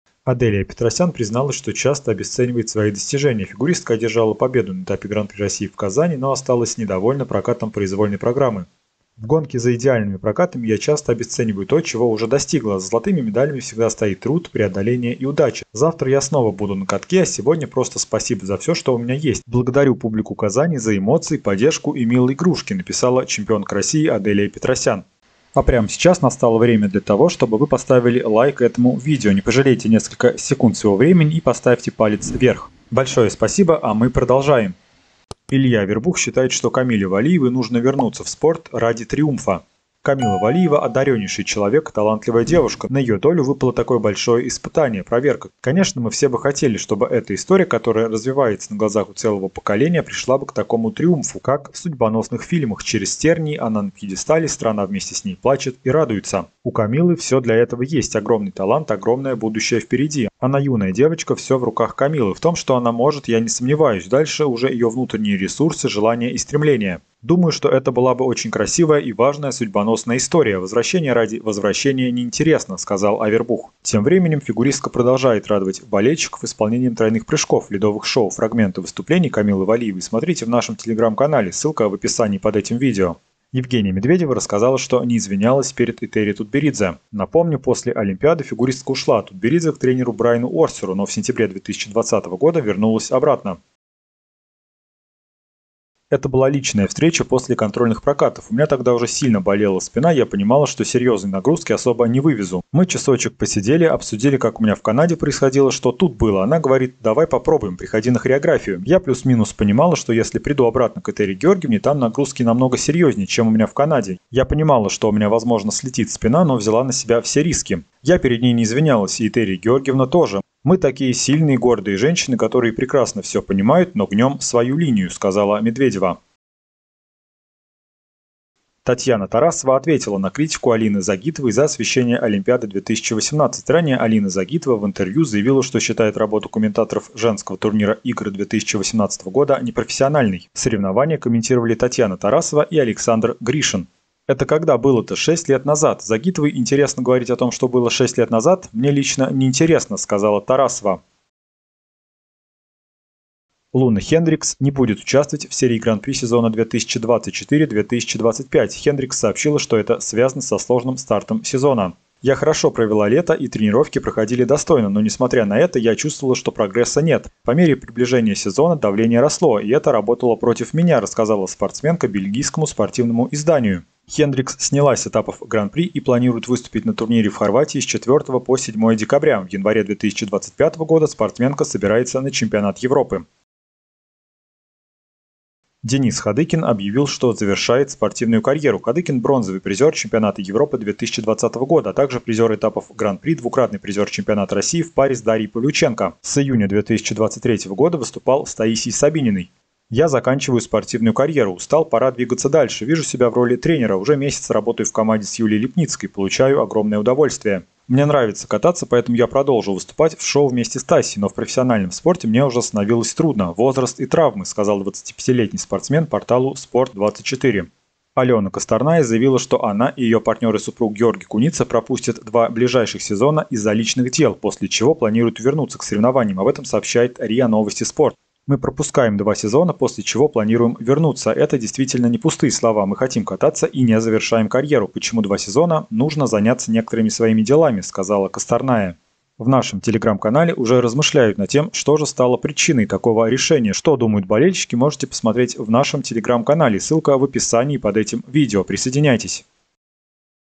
Завершение карьеры. Пропустят 2 сезона. Валиевой нужно вернуться. Снятие Хендрикс
Алена Косторная сообщила, что они с Георгием Куницей пропустят два сезона.
Денис Ходыкин объявил, что завершает спортивную карьеру.
Илья Авербух считает, что Камиле Валиевой нужно вернуться в спорт ради триумфа.
Аделия Петросян призналась, что часто обесценивает свои достижения.
Луна Хендрикс не будет участвовать в серии Гран-при 2024/2025.